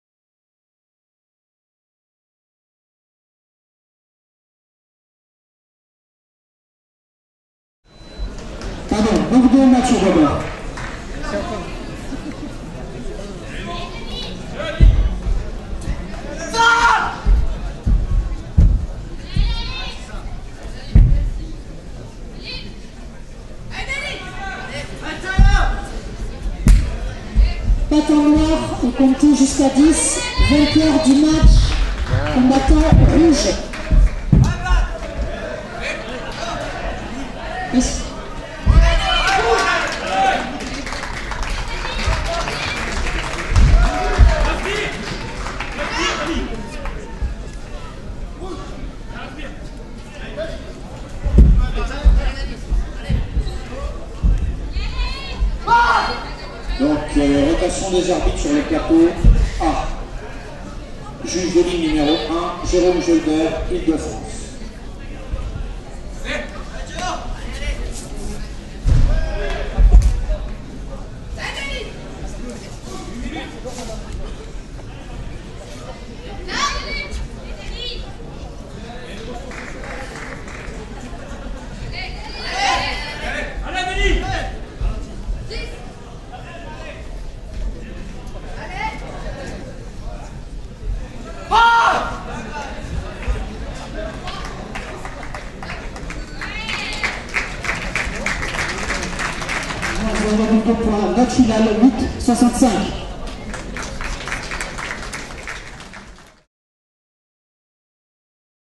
Championnat de France 2012 de Wushu Taolu / Nan Quan Seniors - Dany Perellon
Championnat de France 2012 de Wushu Taolu à Limoges les 11 & 12 février. Epreuve de Nan Quan catégorie masculin Seniors - Dany Perellon